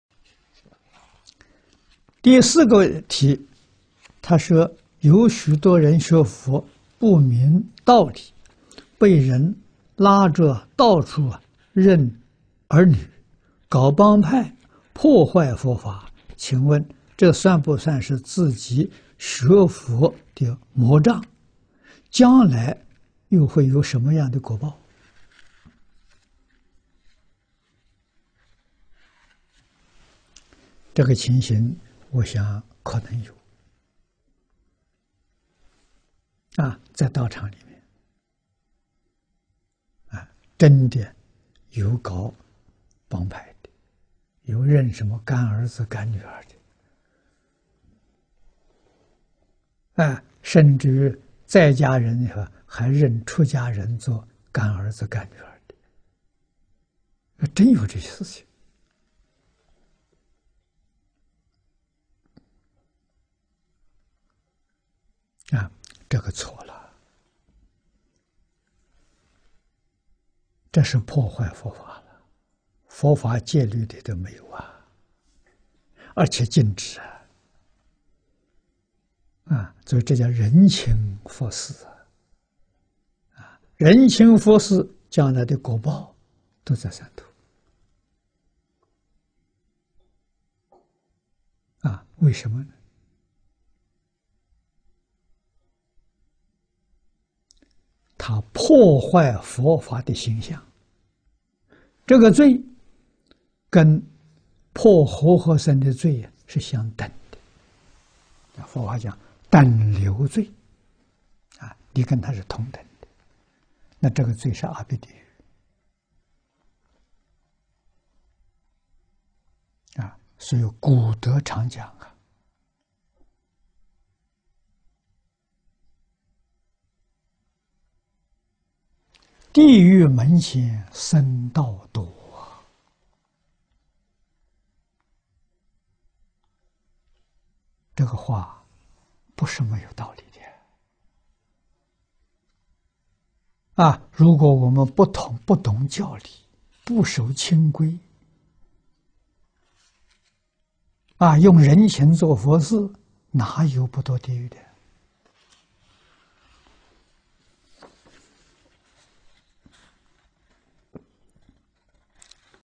淨空老法師:有許多人學佛,不明道理,被人拉著到處認兒女、搞幫派,破壞佛法,請問這算不算自己學佛的魔障？將來又會有什麼樣的果報？
節錄自—學佛答問（答香港參學同修之九十二）
~日期：2008/4/11，（共一集）地點講於:華嚴講堂,香港

影片字幕：
問：第四個題，有許多人學佛，不明道理，被人拉著到處認兒女、搞幫派，破壞佛法。請問這算不算自己學佛的魔障？將來又會有什麼樣的果報？
答：這個情形，我想可能有。在道場裡面，真的有搞幫派的，有認什麼乾兒子、乾女兒的。甚至於在家人裡頭，還認出家人做乾兒子、乾女兒，真有這些事情。這個錯了，這是破壞佛法，佛法戒律裡頭沒有，而且禁止。這叫人情佛事，人情佛事將來的果報都在三途。為什麼？他破壞佛法的形象，這個罪跟破和合僧的罪是相等的，佛法講等流罪，你跟他是同等的，那這個罪是阿鼻地獄。所以古德常講「地獄門前僧道多」，這個話不是沒有道理。如果我們不懂教理，不守清規，用人情做佛事，哪有不墮地獄的！